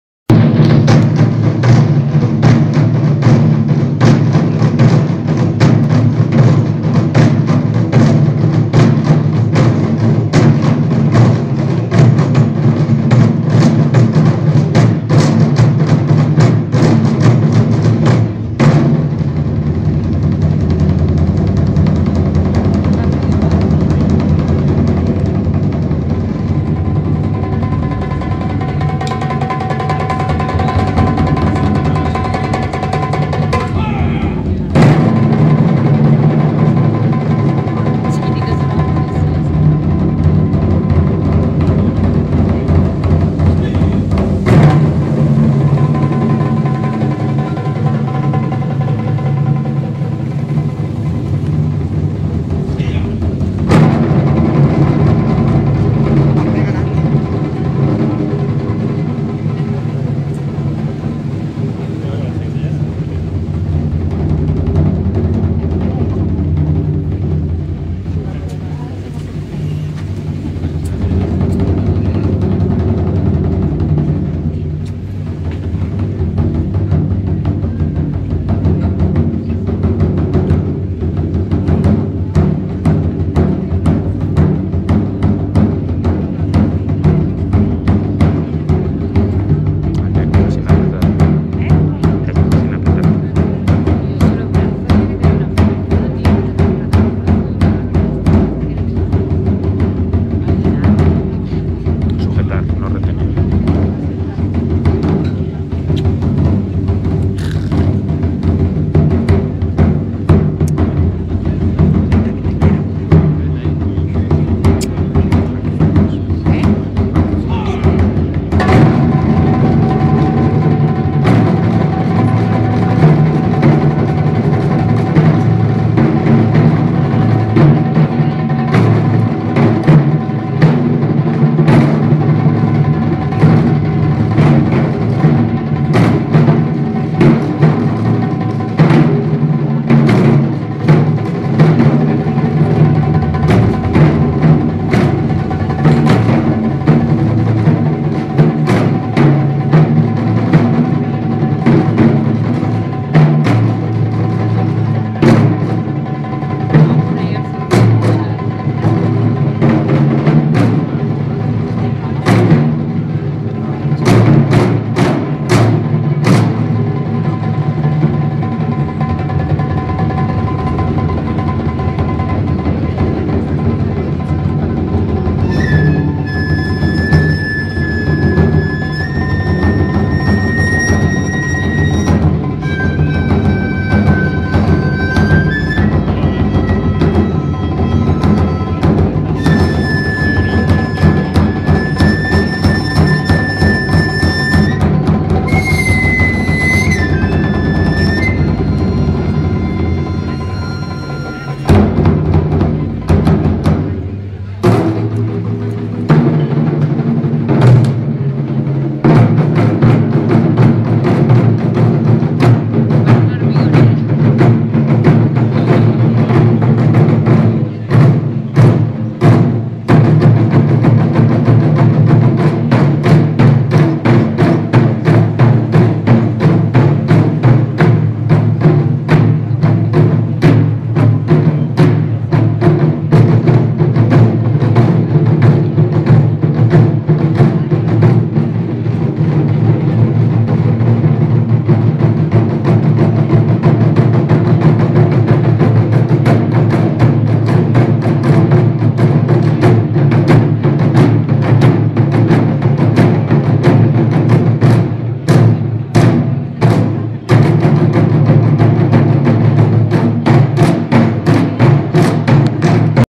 La Noche en Blanco, 2008 (Retiro)
algo de percusión japonesa en el Retiro, por La Noche en Blanco 2008.